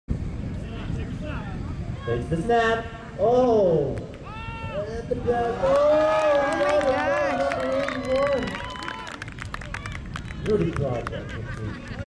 Chel with the Extra Point
B-League Mad Mix Up 2010. Championship game MH vs HP